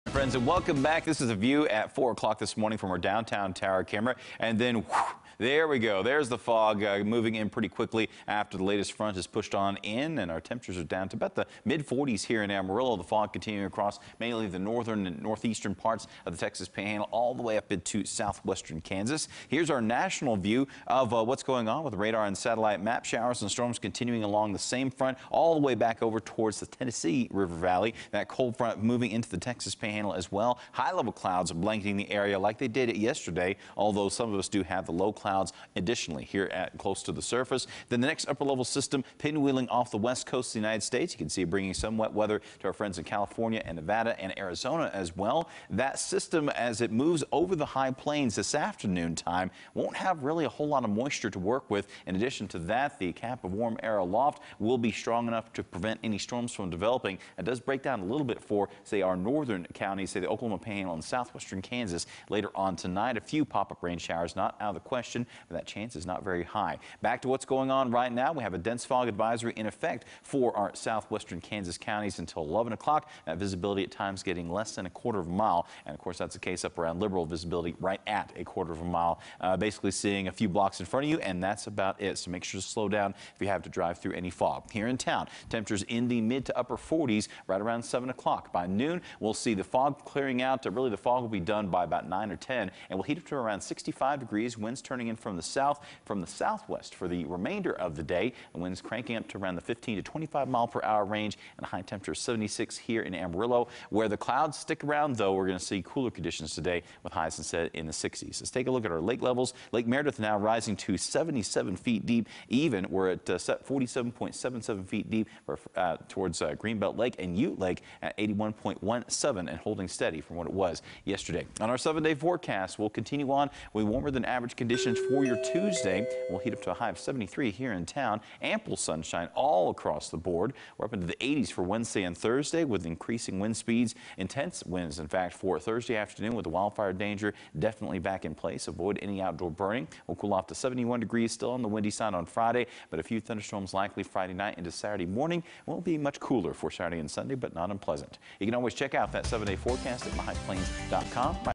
Warm weather continues this week
It's a cool start to what will be a warm day for many locations in the Panhandle, as a cold front that has moved in will be heading up to the northeast.